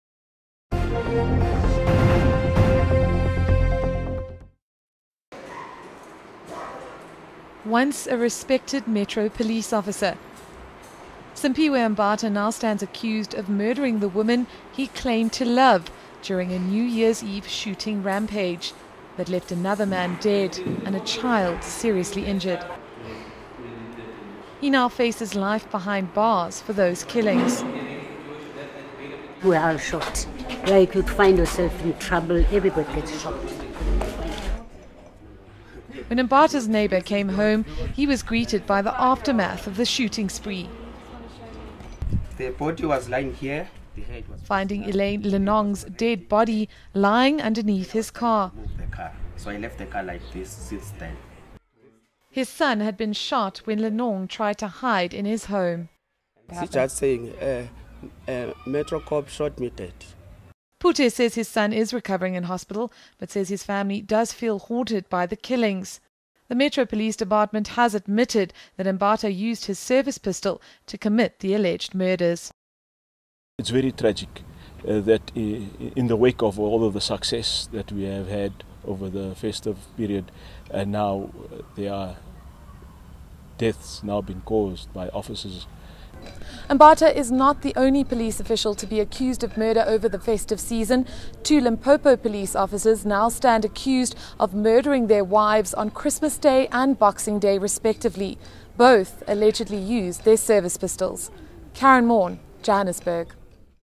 Metro Cop Accused of Murder
Johannesburg, January 3 - A Metro police officer stands accused of two murders and four attempted murders. After he allegedly kidnapped his ex-girlfriend Simphiwe Mbatha and went on a shooting rampage on New Year's Eve.